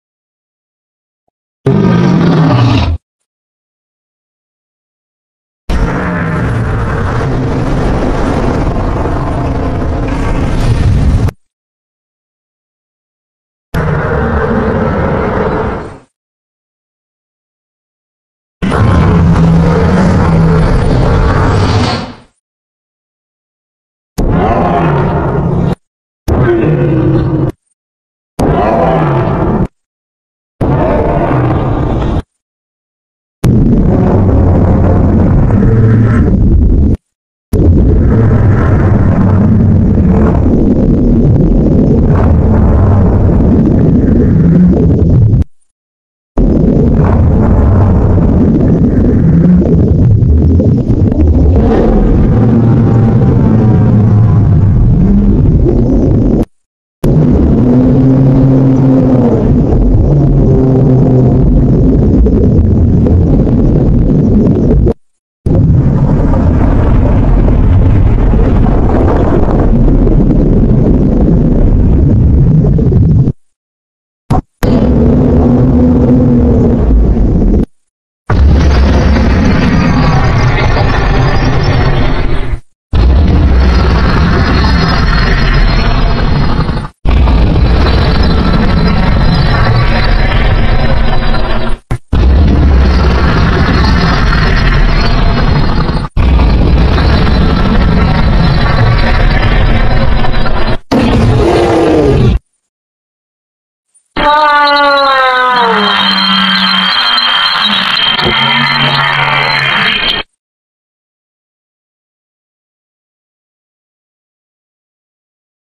Amphibian Diplocaulus Sounds
Diplocaulus is an extinct genus of leopospondyl amphibians which lived from the Late Carboniferous to the Late Permian of North America and Africa